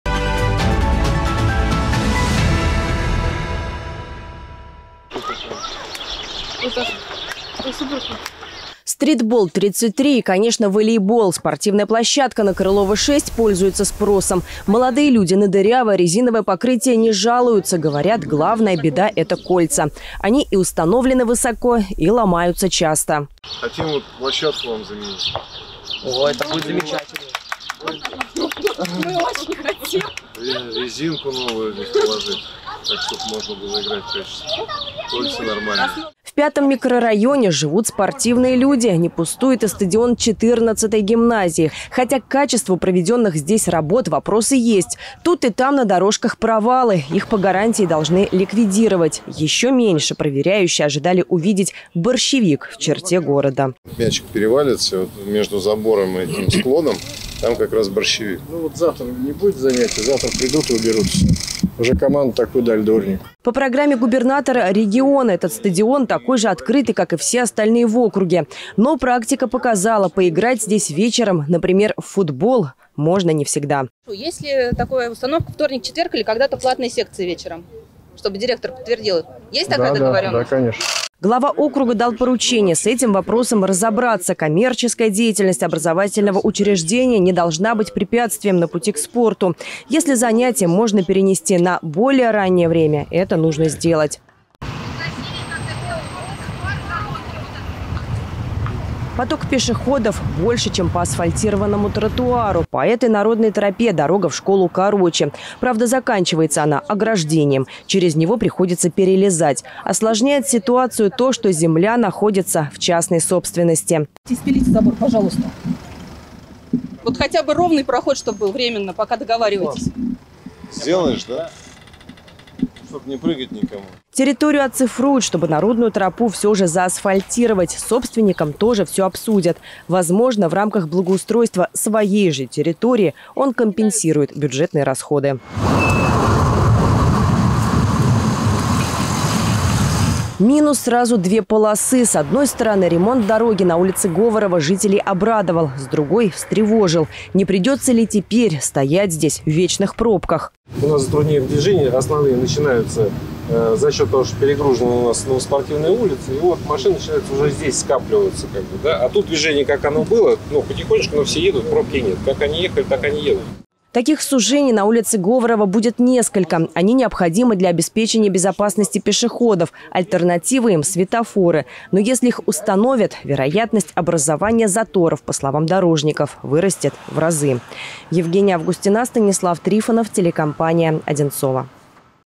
Велоинспекция 5 микрорайона
Доступность «открытого» стадиона, ремонт спортивной площадки, сужение проезжей части и провалы на пешеходных дорожках. Это лишь часть вопросов, которые подняли жители 5-ого микрорайона Одинцово на велоинспекции с главой округа. Обо всем по порядку - расскажем.